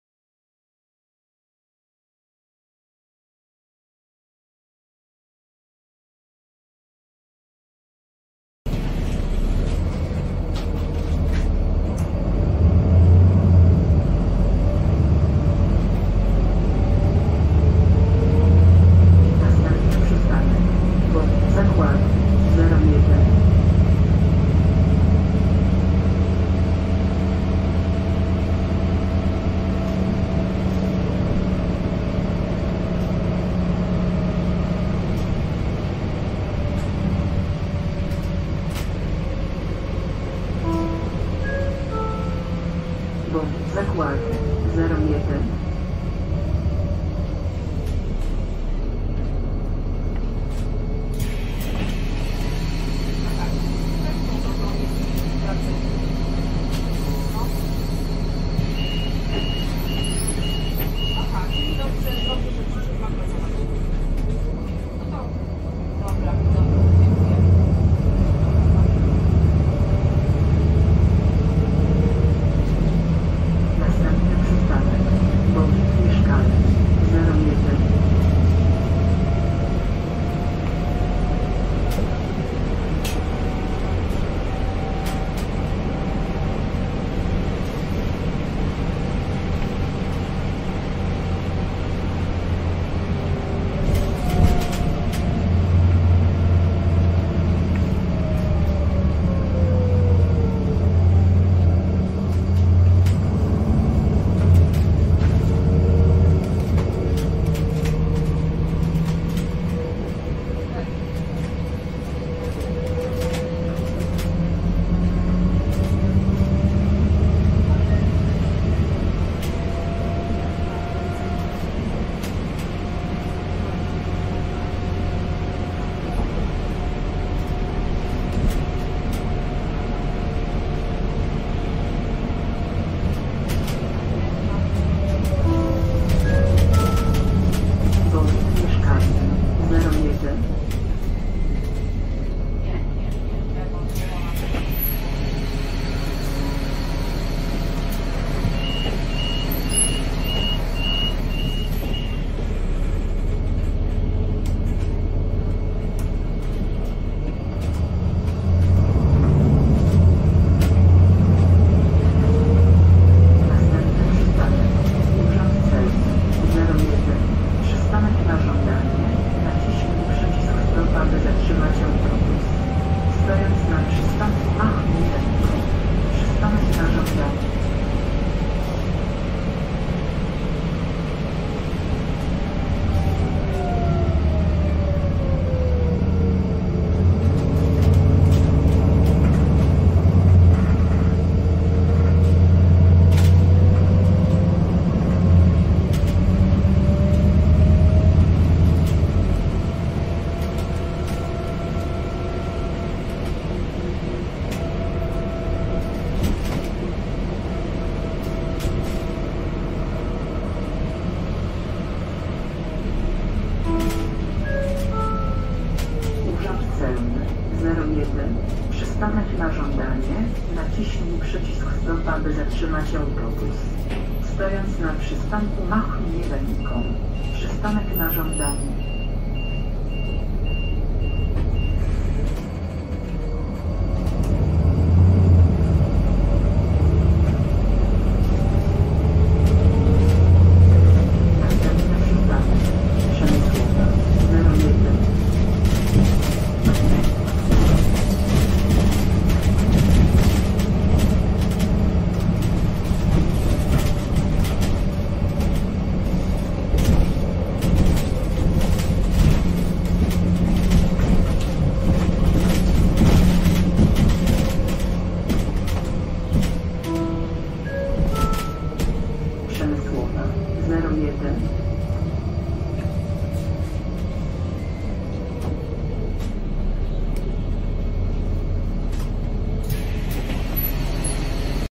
MZK Koszalin - Scania CN280UB Citywide #2046, Linia 10
Scania Citywide to następca produkowanej od 1997 roku Scanii Omnicity oraz Omnilink. Koszalin, którego tabor autobusów miejskich składa się obecnie  głównie ze szwedzkiej marki, swoje pierwsze cztery autobusy typu Omnicity otrzymał w 2006 roku. Było to duże zaskoczenie, gdyż prym w Koszalinie wiódł wówczas MAN i chyba każdy spodziewał się wtedy pierwszych żółtych Lion'sów, które dopiero co weszły do produkcji. Po 16 latach od dostawy pierwszych Scaniulek widać, że Koszalin bardzo się polubił z niedalekim zakładem produkcyjnym ze Słupska i z biegiem czasu do żółtej floty trafiały coraz to kolejne pojazdy. Zarówno używane, jak i fabrycznie nowe. Ostatnimi nabytkami jest 8 używanych Omnicity z Jaworzna. MZK Koszalin posiada łącznie 35 autobusów od Scanii, w tym 7 sztuk  modelu Citywide z 2018 roku.
Ciekawostką jest, że wersja Omnicity od początku produkcji oznaczana była  jako typ CN94UB, a od wersji z silnikami o normie spalin Euro IV jako CN230UB lub CN270UB.

Linia: 10
Kierunek: CHAŁUBIŃSKIEGO
Przystanki:
1. BoWiD 02
2. BoWiD Zakłady
3. BoWiD Mieszkalny
4. Urząd Celny n/ż
5. Przemysłowa